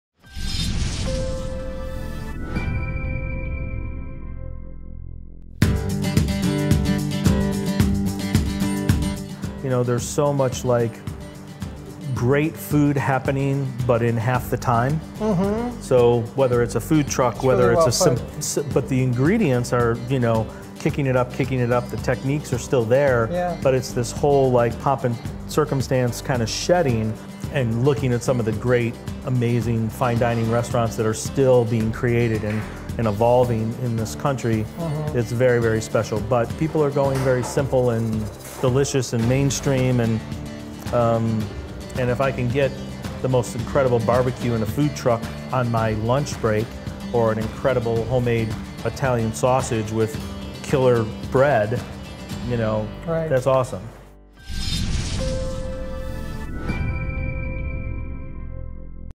Vollrath Chef Series: Rick Tramonto - Great Food in Half the Time
An interview with Rick Tramonto of Tru in Chicago, Ill. (2018-19 Madison College Chef Series presented by Vollrath).